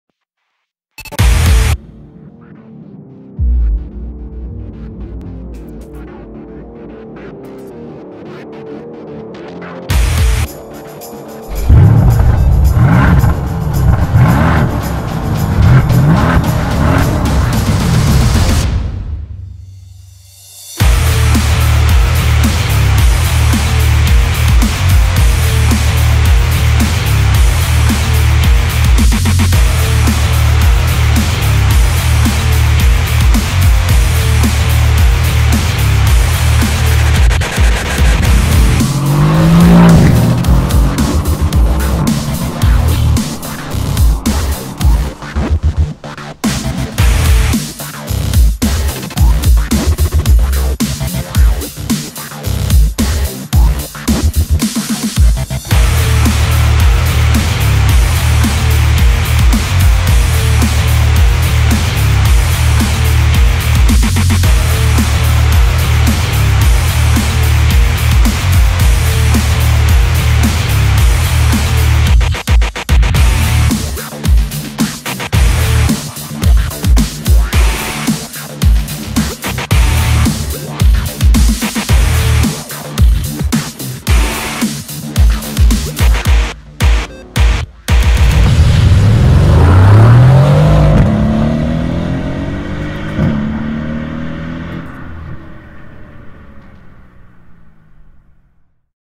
CAR WRAPPING - Mercedes-AMG C63
CAR WRAPPING & CAR STYLING
Jetzt mal wieder ein Highlight. Ein Fahrzeug, wo selbst einem Folierer oder Motorsportfan der Atem stockt. :-)  Vollfolierung und Styling eines Mercedes-AMG C63. Premium Folien von 3M Matte Dark Grey, Gloss Lemon Sting und Satin Black. "Denn glauben wir AMG, dann ist Gelb das neue Rot!" (Zitat: Autobild)